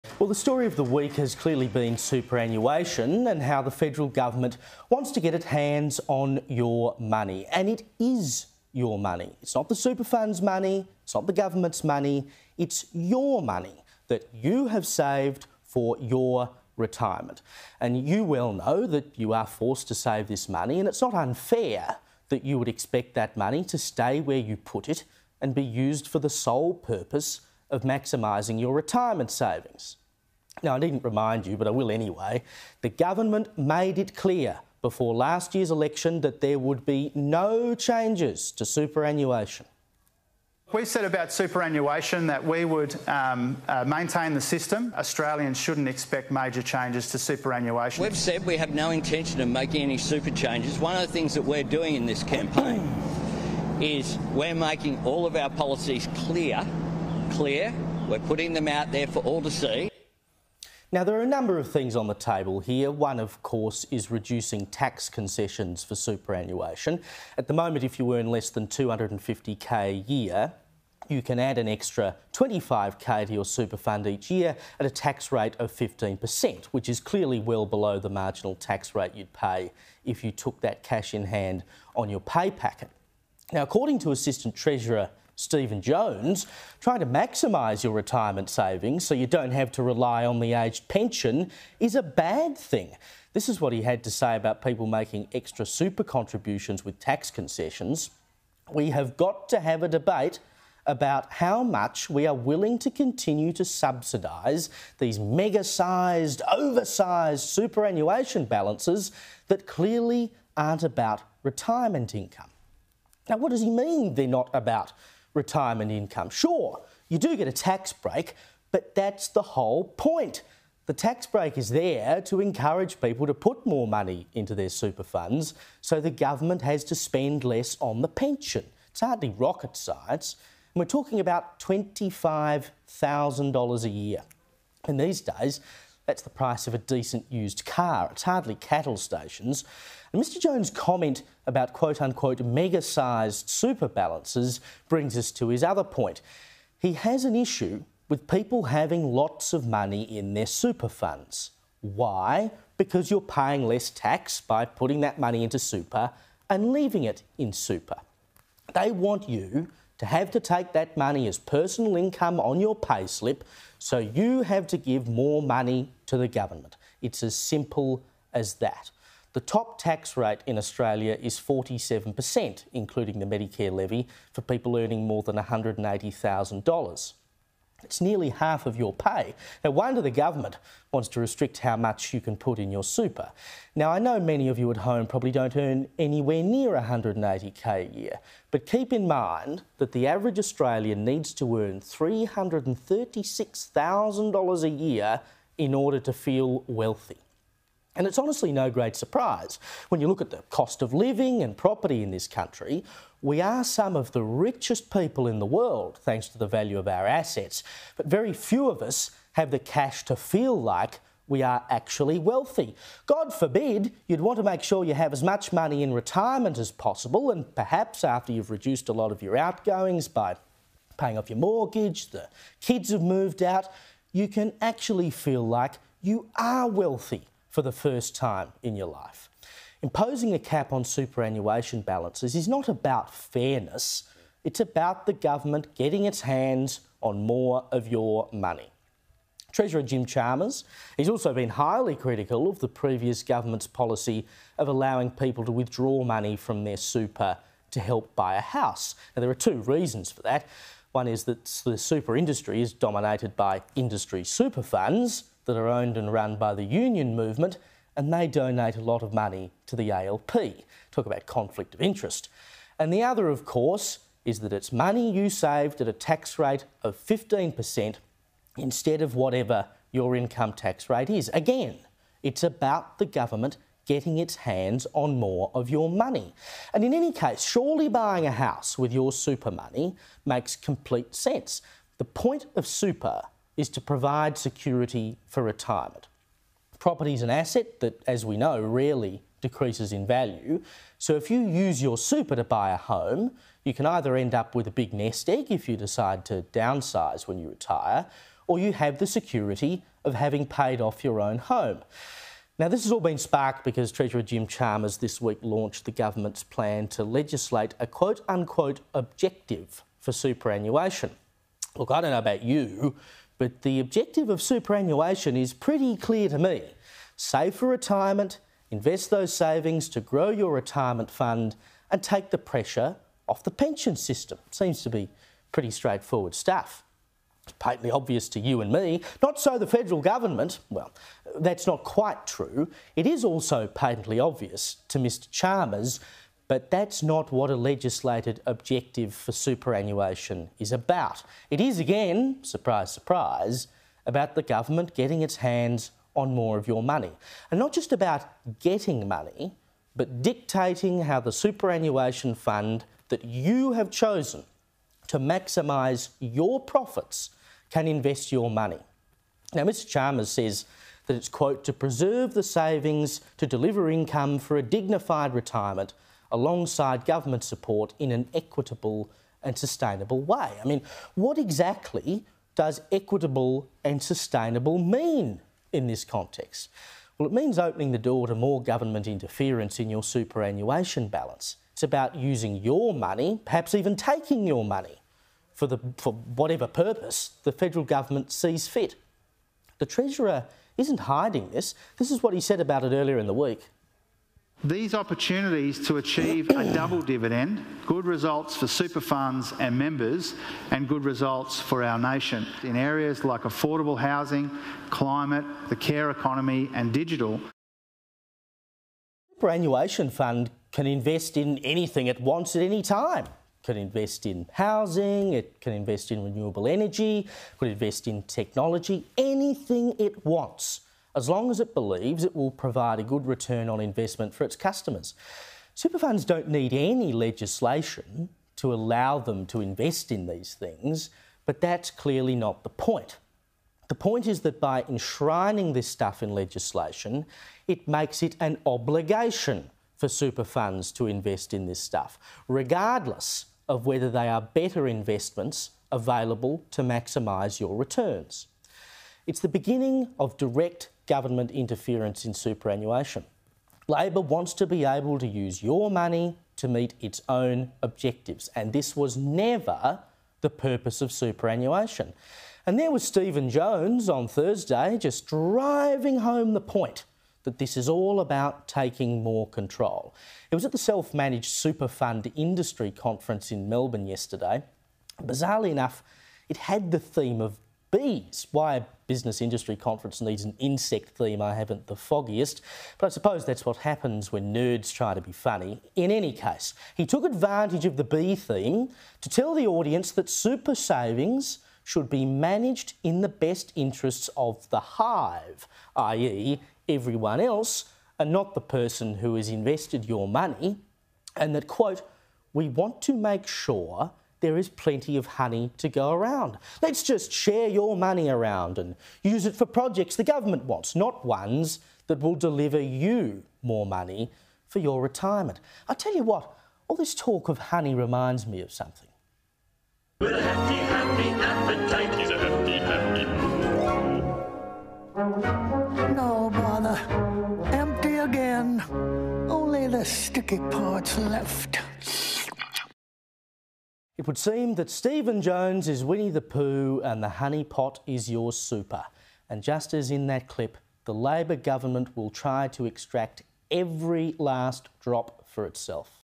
Labor wants to ‘get its hands on your money’: Caleb Bond
The story of the week has been superannuation and how the federal government wants to “get its hands on your money”, says Sky News host Caleb Bond.

Mr Bond said the changes revealed this week are the beginning of “direct government interference in superannuation".

“Labor wants to be able to use your money to meet its own objectives and this was never the purpose of superannuation," Mr Bond said.